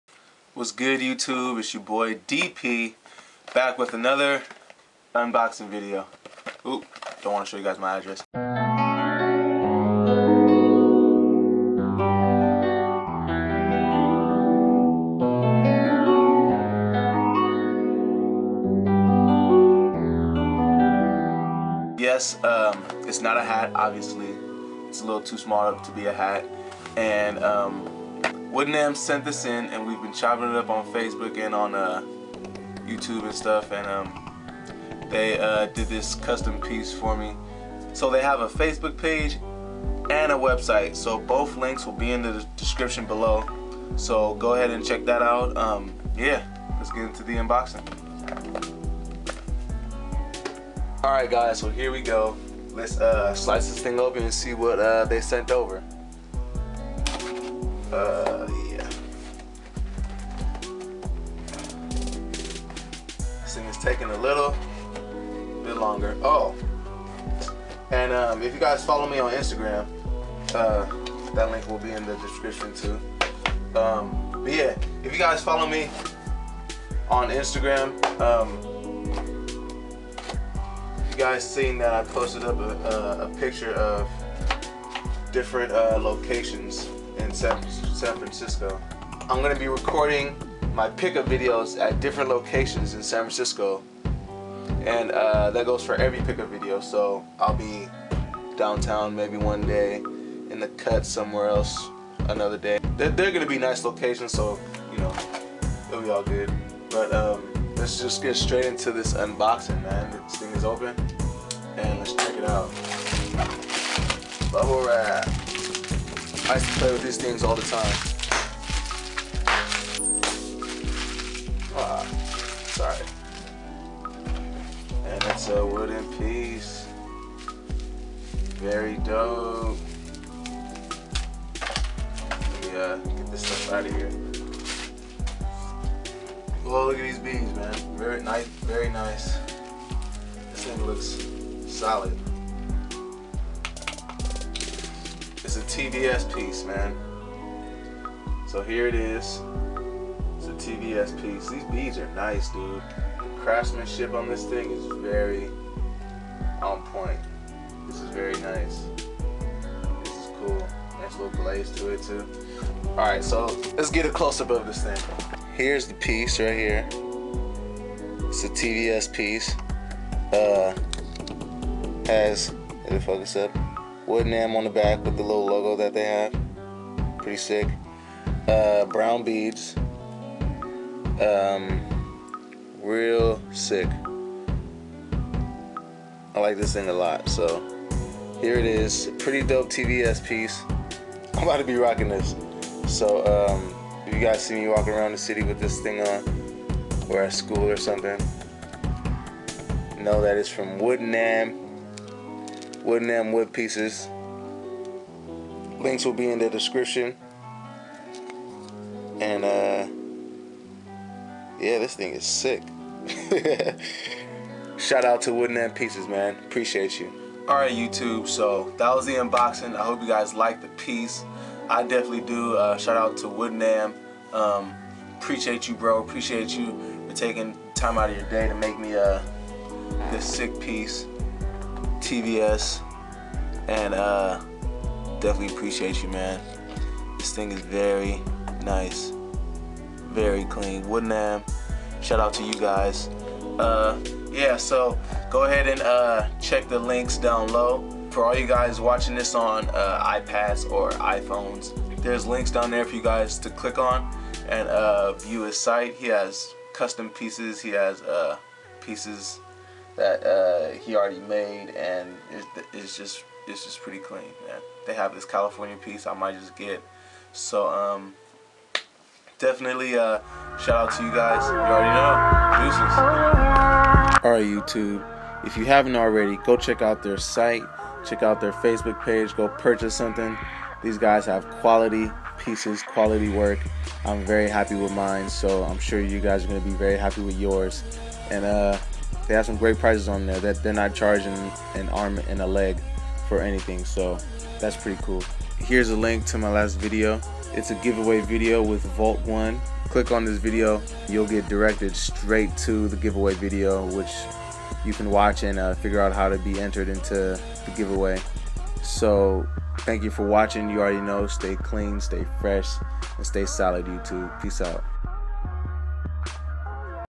UNBOXING with WOODNAM my first wood piece!!!
WHATS UP YOUTUBE!! I was contacted by the good folks up at WoodNam and they sent me this Great looking wooden piece. Its my first wooden piece ever but I'm definitely going to be buying allot more. So sit back and check out this great piece that they sent me
Thank you to all my SUBS and you new comers lol